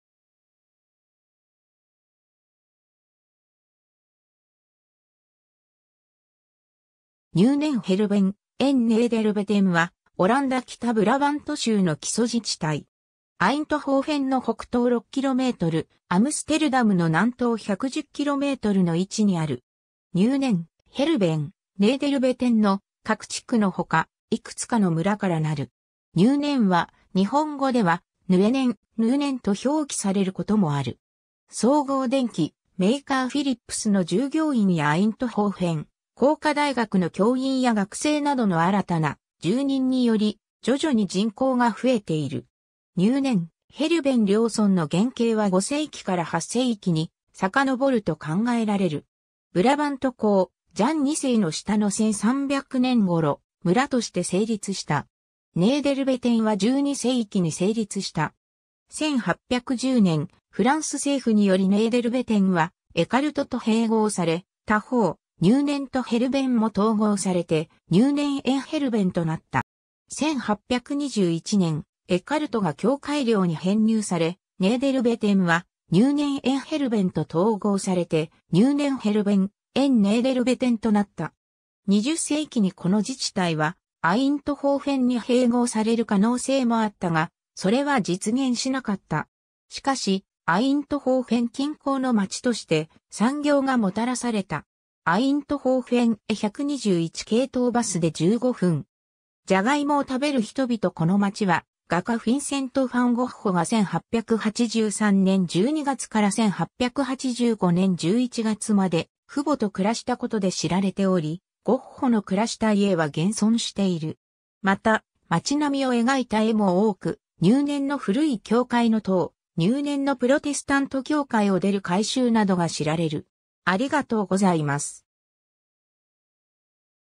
ニューネン・ヘルヴェン・エン・ネーデルヴェテン
ニューネン・ヘルヴェン・エン・ネーデルヴェテン（Nuenen, Gerwen en Nederwetten [ˌnynə(n) ˌɣɛrʋə(n) ɛn neːdərˈʋɛtə(n)] ( 音声ファイル)）は、オランダ北ブラバント州の基礎自治体（ヘメーンテ）。
アイントホーフェンの北東6km、アムステルダムの南東110kmの位置にある。
ニューネン、ヘルヴェン、ネーデルヴェテンの各地区のほか、いくつかの村（オプウェッテン、ボールト、エーンアイント、スタト・ファン・ヘルヴェン、オレン）から成る。
 「ニューネン」は、日本語では「ヌエネン」「ヌーネン」と表記されることもある。
 総合電機メーカーフィリップスの従業員やアイントホーフェン工科大学の教員や学生などの新たな住人により徐々に人口が増えている。
 ニューネン、ヘルヴェン両村の原形は5世紀から8世紀に遡ると考えられる。
ブラバント公ジャン2世の下の1300年頃、村として成立した。
ネーデルヴェテンは12世紀に成立した。
1810年、フランス政府によりネーデルヴェテンはエカルトと併合され、他方、ニューネンとヘルヴェンも統合されてニューネン・エン・ヘルヴェンとなった。
1821年、エカルトが教会領に編入され、ネーデルヴェテンはニューネン・エン・ヘルヴェンと統合されてニューネン・ヘルヴェン・エン・ネーデルヴェテンとなった。
 20世紀にこの自治体はアイントホーフェンに併合される可能性もあったが、それは実現しなかった。
しかしアイントホーフェン近郊の町として、産業がもたらされた。
 アイントホーフェンへ121系統バスで15分（1時間に6本運行）。
 ジャガイモを食べる人々 （フィンセント・ファン・ゴッホ作 1885年） この街は、画家フィンセント・ファン・ゴッホが1883年12月から1885年11月まで父母と暮らしたことで知られており、ゴッホの暮らした家は現存している。
また、街並みを描いた絵も多く「ニューネンの古い教会の塔」、「ニューネンのプロテスタント教会を出る会衆」などが知られる。